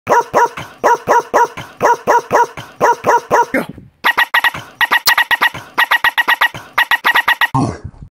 The Best Poodle Beat You've Heard Today
It's short. It's stupid. But its the best poodle beat you've heard today. (If someone wants to loop this for me that'd be rad)